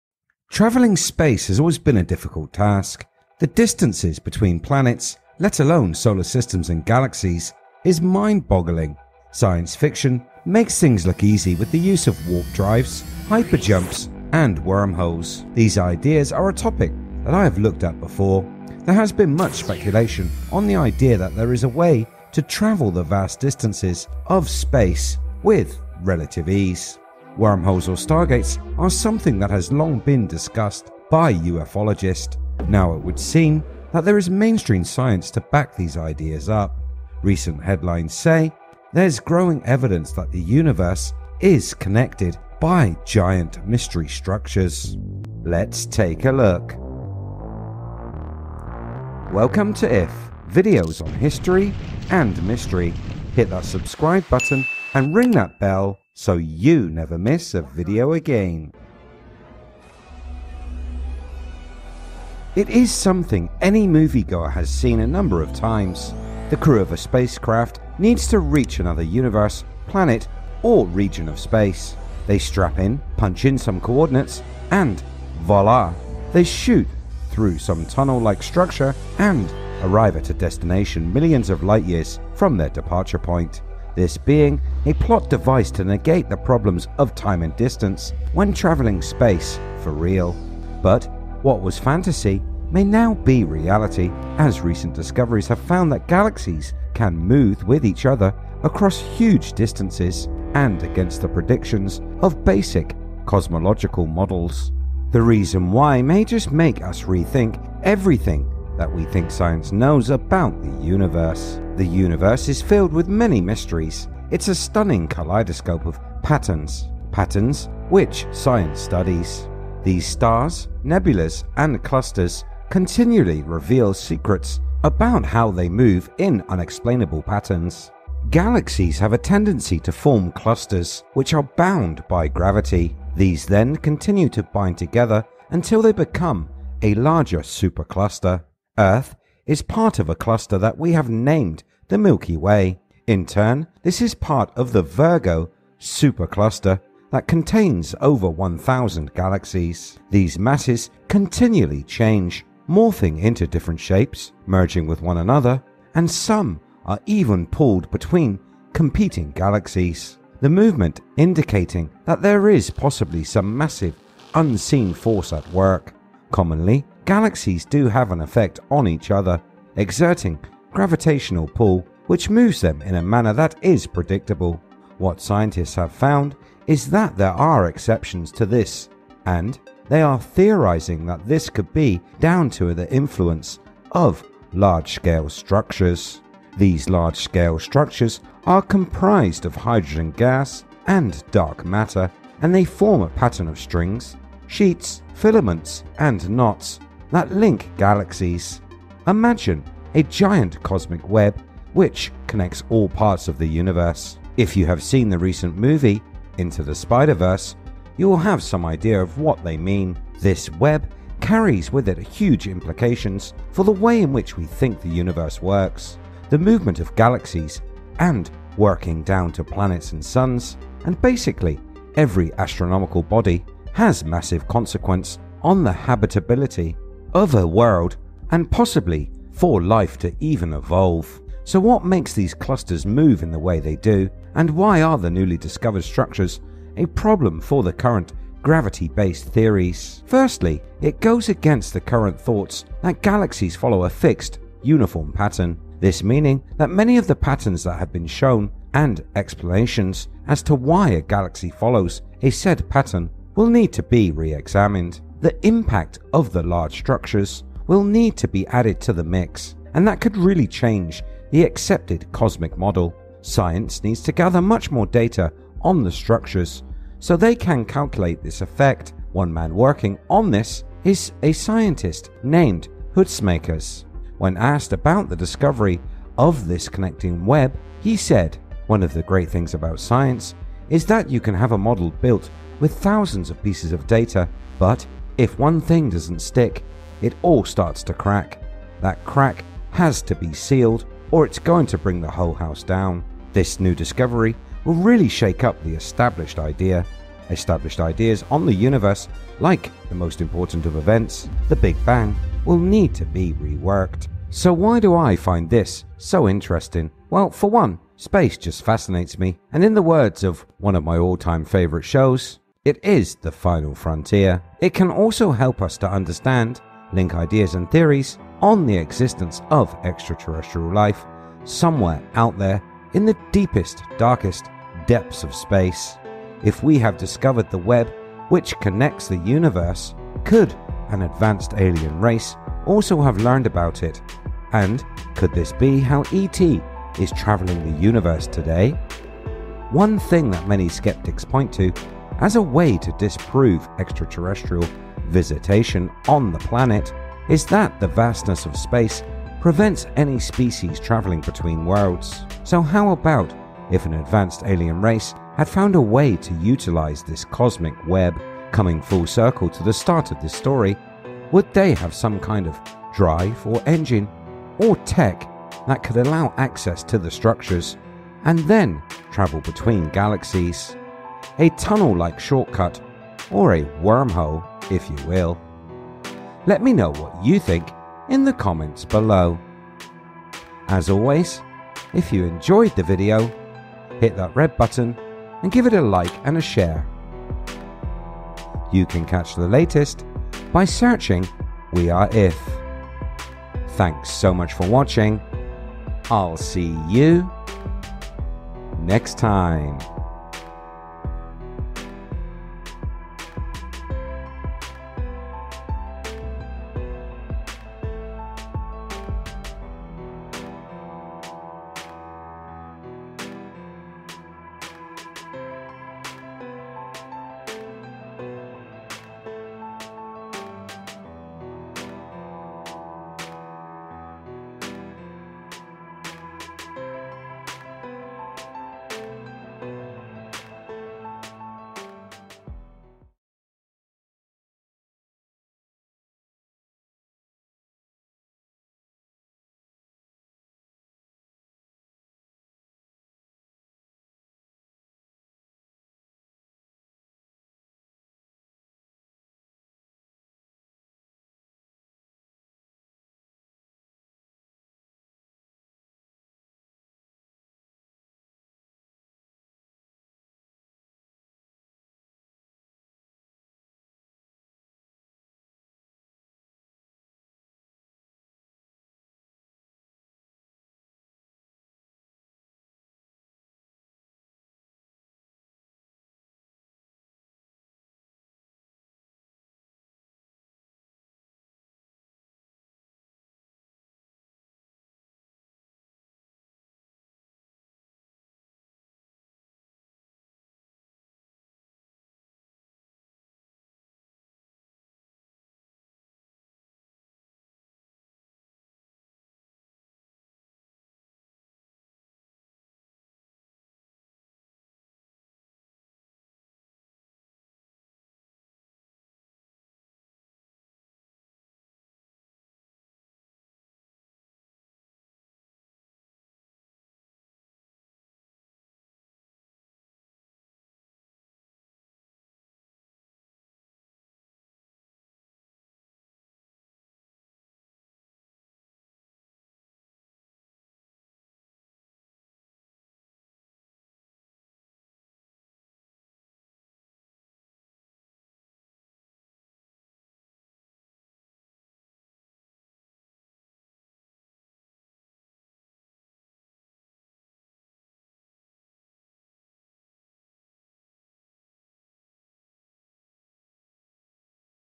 HOW TO TRAVEL THE UNIVERSE - Extraterrestrials and the cosmic web. 👽👽👽
Extraterrestrials and the cosmic web.

Traveling space has always been a difficult task the distances between planets let alone solar systems and galaxies is mind boggling.
Science fiction makes things look easy with the use of warp drives, hyper jumps and worm holes. These ideas are a topic that I have looked at before, there has been much speculation on the idea that there is a way to travel the vast distance of space with relative ease.
Wormholes or star-gates are something that has long been discussed by ufologist, now it would seem that there is mainstream science to back these ideas up.
Recent headlines said there’s Growing Evidence That the Universe Is Connected by Giant Structures.

IF The only question we ever need.

Creating  YouTube content to intrigue, fascinate and amuse.

Speculation, education and imagination. Wondering how what or 'IF'

"many thanks"